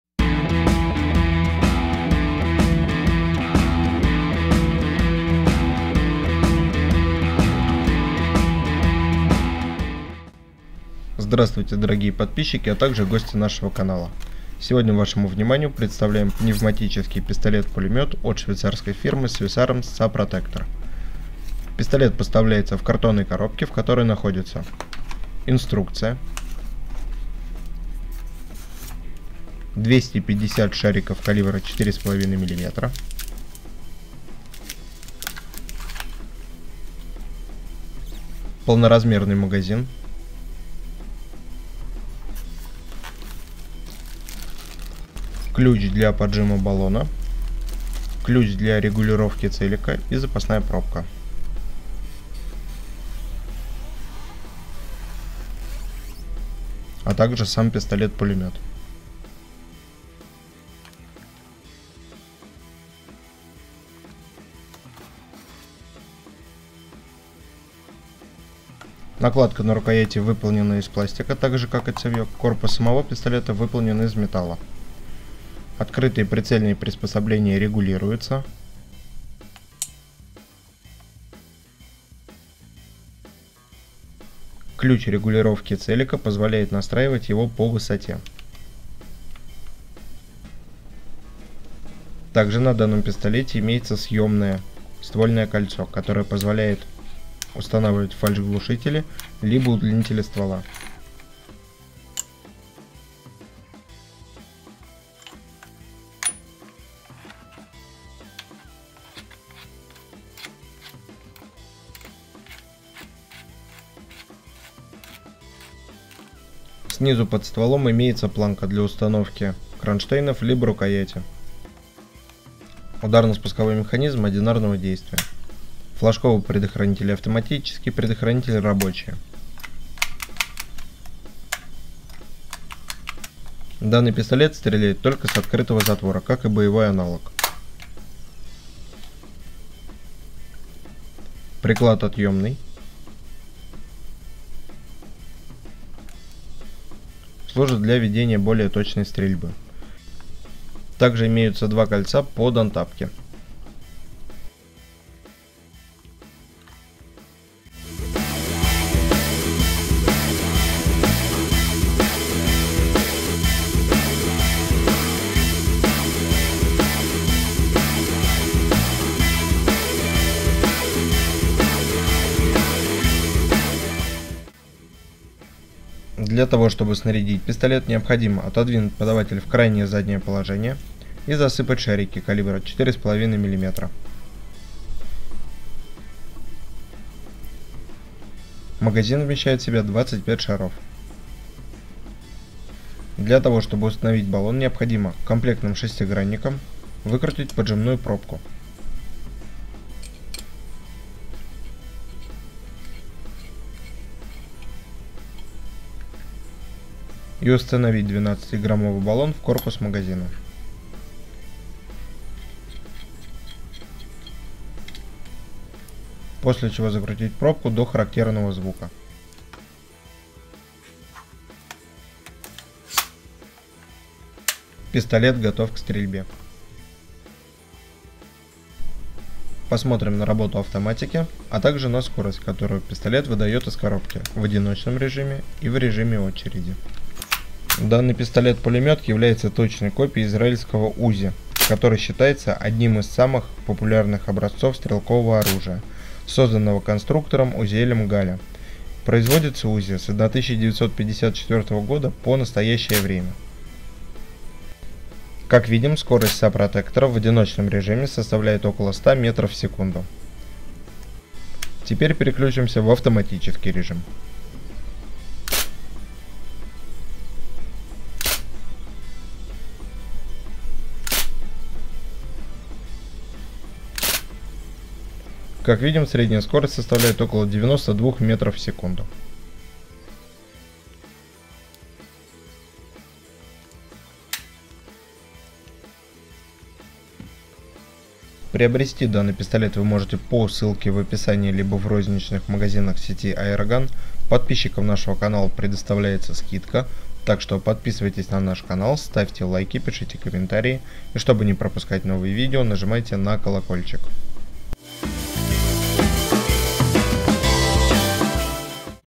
Пневматический пистолет-пулемет Swiss Arms SA-PROTECTOR 4,5 мм
Огнестрельный оригинал был разработан специально для телохранителей и служб сопровождения высокопоставленных лиц, для этого он сделан весьма компактным, но, при этом, мощным и удобным в использовании. Пневматическая реплика обладает практически всеми теми же качествами, что и оригинал.

В базовой компоновке пуля при вылете из ствола имеет энергию менее 3 Дж, что подтверждается официальным сертификатом соответствия. Благодаря этому Swiss Arms SA-PROTECTOR купить можно без лицензии.